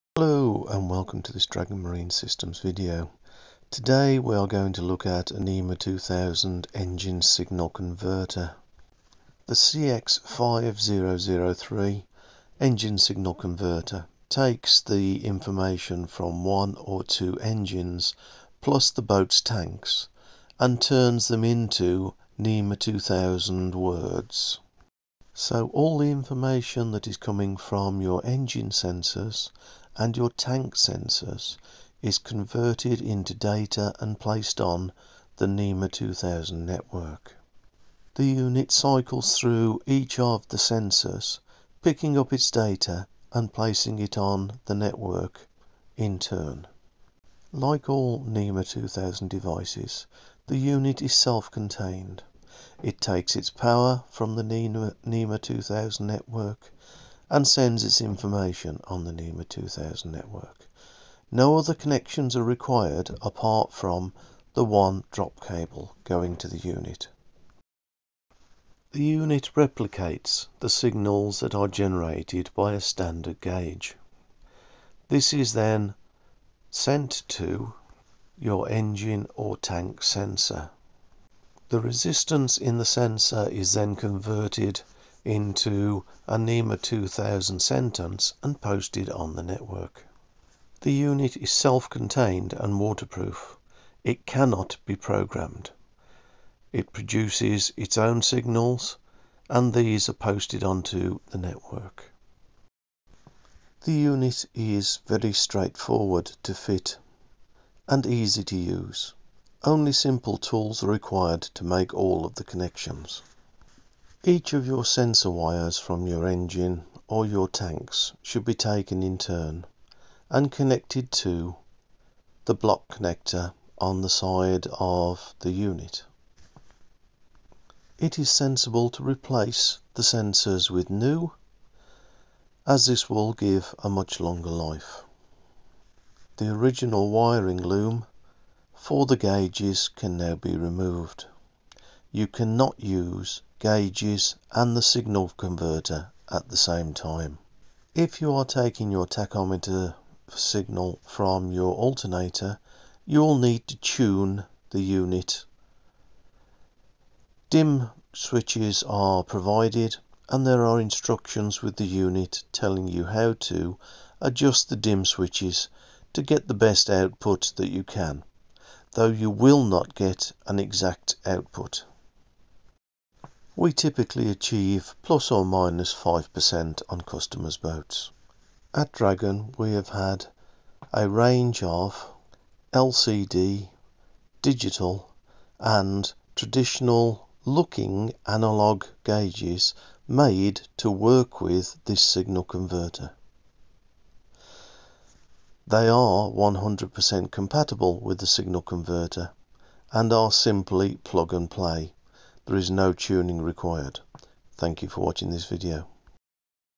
CX5003 NMEA 2000 engine signal converter
In this video we look at taking all the information from your engine and tanks and putting it on the NMEA 2000 network.